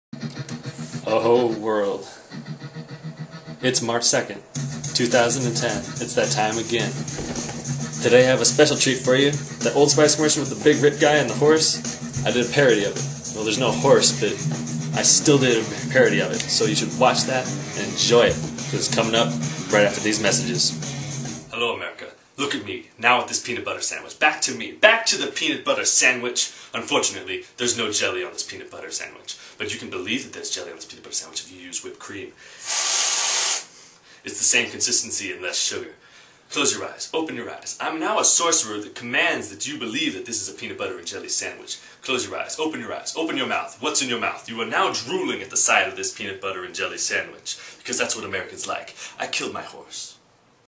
Day 61 of 2010 - Old Spice Commercial Parody
You know you want to comment on this one. I love the Old Spice commercials right now. I'm on a horse? Genius. I took the time today to do my own little version of those commercials and I'm pleased with how it turned out. Really wasn't that hard to do at all! I am the man your man could smell like. Enjoy it, world!